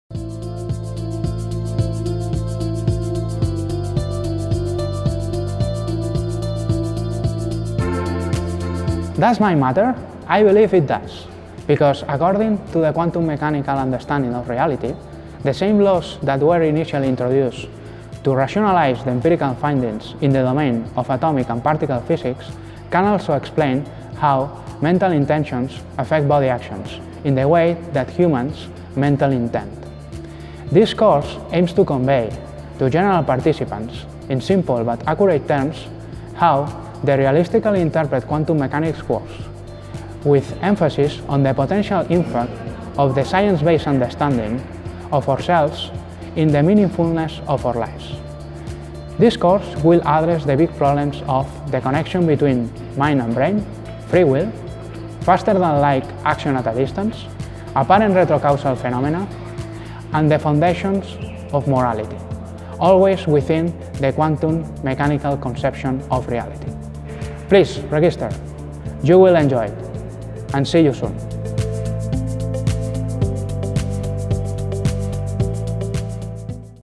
Does Mind Matter? Mind, Matter and Quantum Mechanics - BISS 2019
This course aims to convey to general participants’, in simple but accurate terms, how the realistically interpreted quantum mechanics works, with emphasis on the potential impact of this science-based understanding of ourselves on the meaningfulness of our lives, and on our bringing into being that which we positively value. According to the quantum mechanical understanding of reality, the same laws that were introduced initially to rationalize the empirical findings in the domain of atomic and particle physics also explain how mental intentions can affect body actions in the way that a person mentally intends.